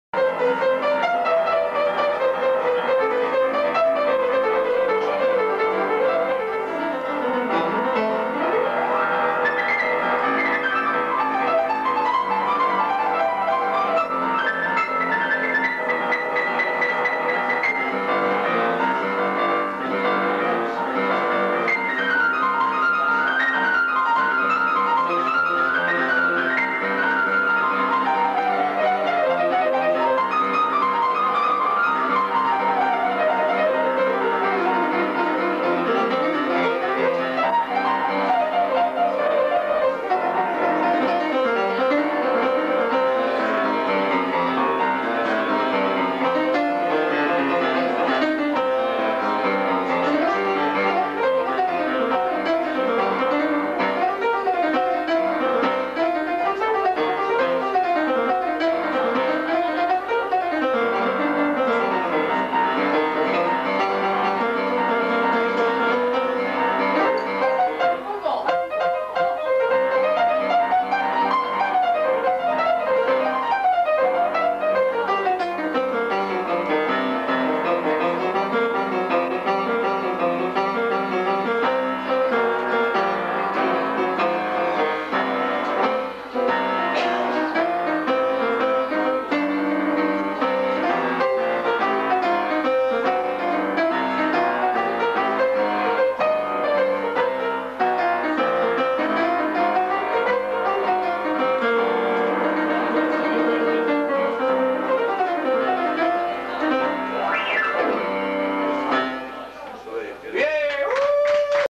PIANO IMPROVISACION, LECCION 2-PIANO IMPROVISATION, LESSON 2. Impro Flamenco Jazz Blues Valladolid
PIANO IMPROVISACION: En la lección 1 aprendimos la diferencia entre teclas blancas negras, y el número de octavas del piano. Hoy aplicaremos esos conocimientos repitiendo este sólo improsado por el pianista.
PIANO IMPROVISATION: In Lesson 1 we learned the difference between black white keys and the number of octaves of the piano. Now apply this knowledge by repeating this just impro with piano